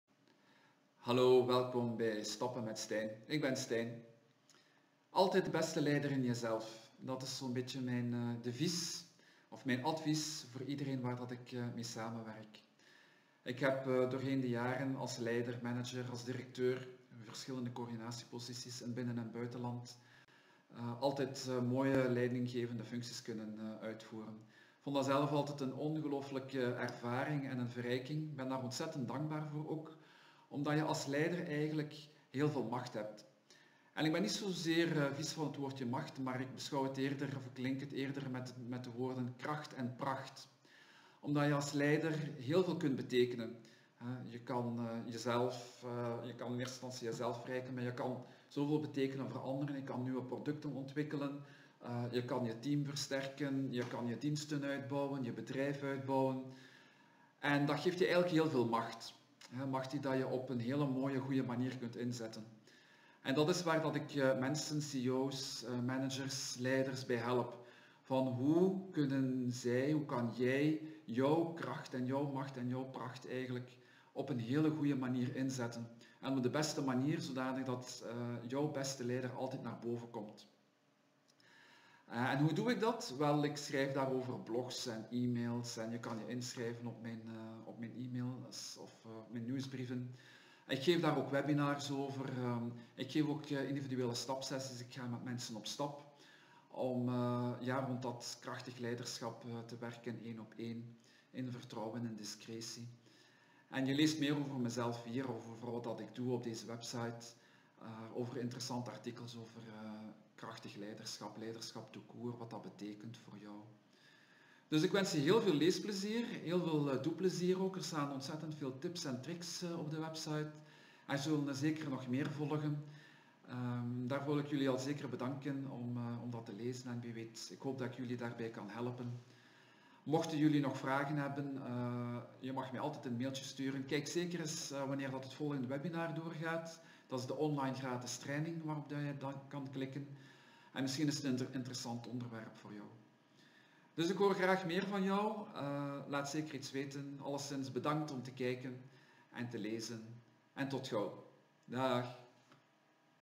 Stappen met Stijn
In deze video vertel ik je kort wie ik ben en wat ik doe met mijn bedrijf Stappen met Stijn.